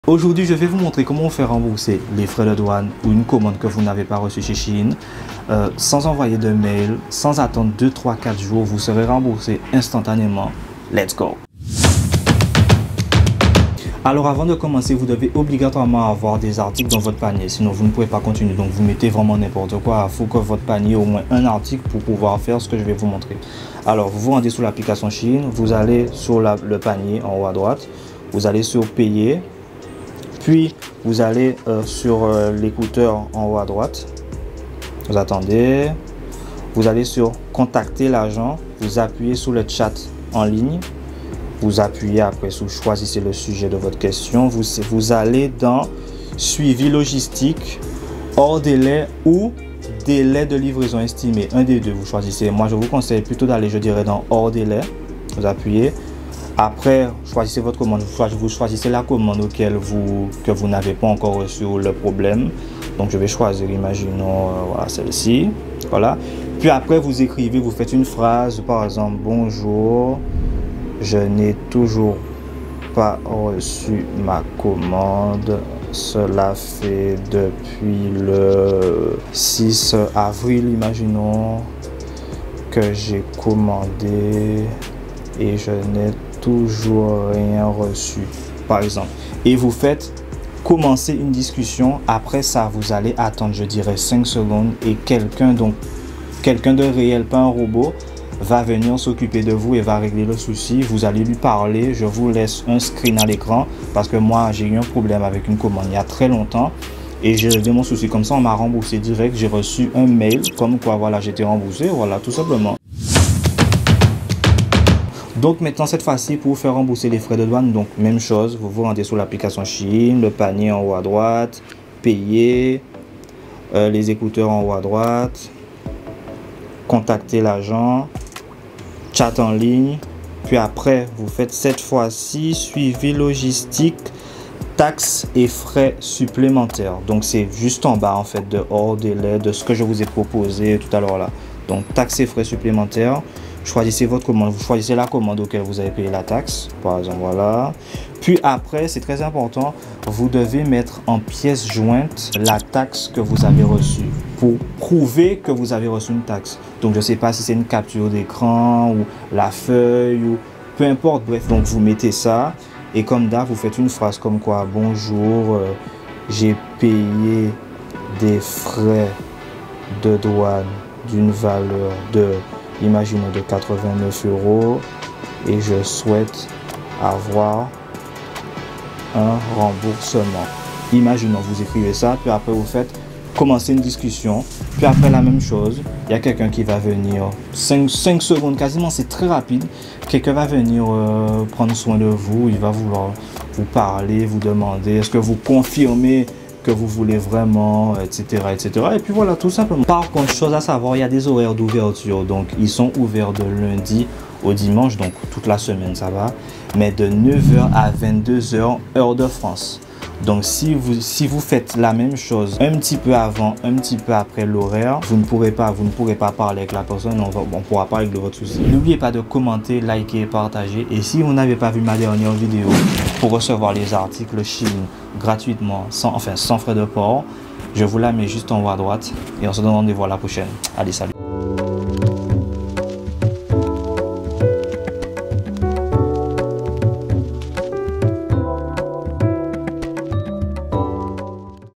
COMMENT SE FAIRE REMBOURSER TRES RAPIDEMENT LA DOUANE OU UNE COMMANDE SUR SHEIN
Dans cette vidéo je vous montre comme se faire rembourser une commande SHEIN et aussi comment se faire rembourser les frais de douane sur le site de SHEIN
comment se faire rembourser sur l'application she in * sur le site la manip n'est pas disponible !

⏰ Horodatages :
00:00 Introduction
00:11 comment se faire rembourser sur l'application she in
01:53 comment se faire rembourser les taxes sur l'application she in
03:19 INFORMATION IMPORTANTE !

JOURS DE PUBLICATIONS DE MES VIDEOS 🖥 📊